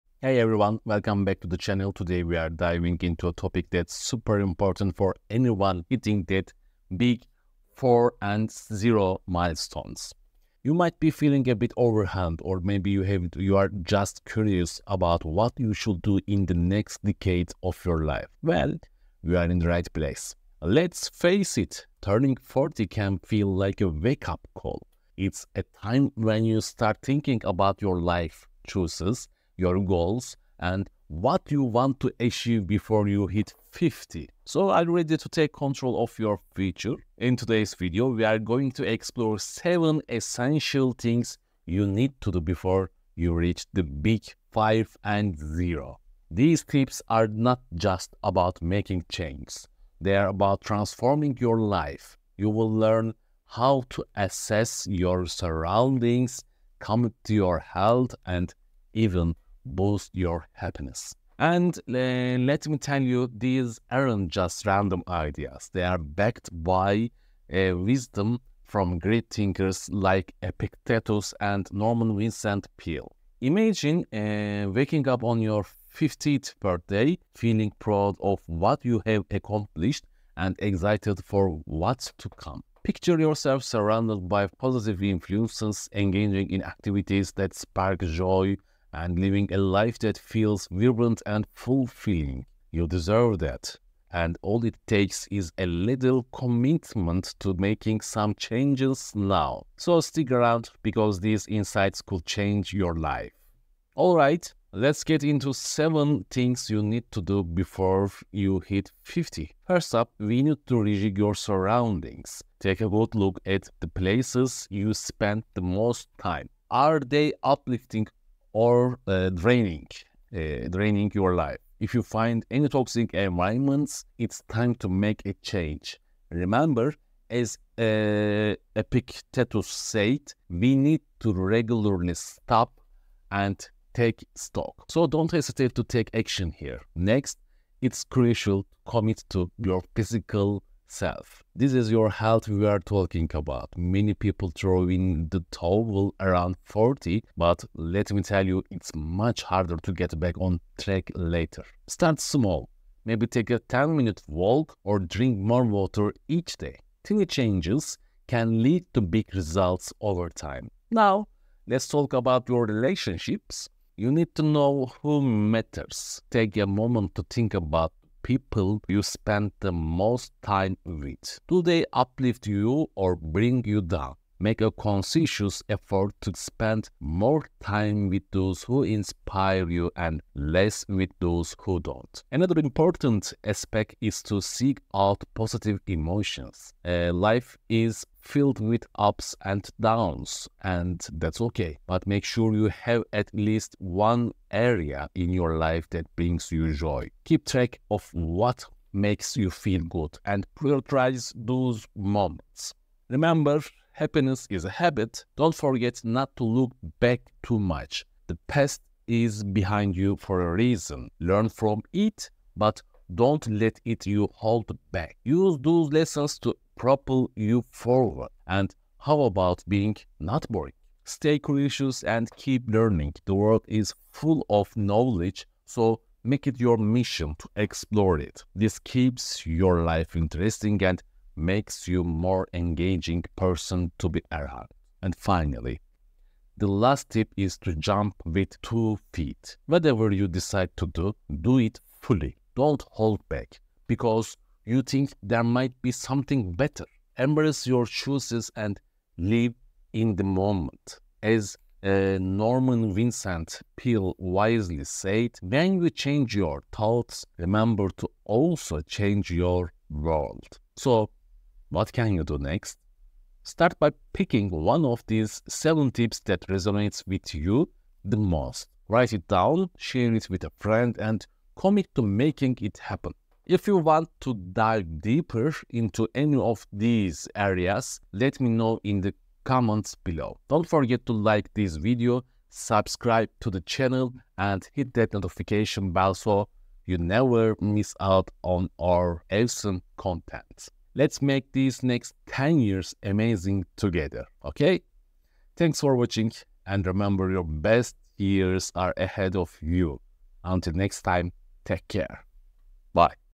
If you are over 40, you should do...!
If you are over 40's, lets talk!

Turning 40 can feel like a wake-up call. It’s a time when you start thinking about your life choices, your goals, and what you want to achieve before you hit 50. So, are you ready to take control of your future?

“When you change your thoughts, remember to also change your world.”

So what can you do next? Start by picking one of these seven tips that resonates with you the most. Write it down, share it with a friend, and commit to making it happen.

🌍 You can check out the videos below to learn more about the places I visit and to support: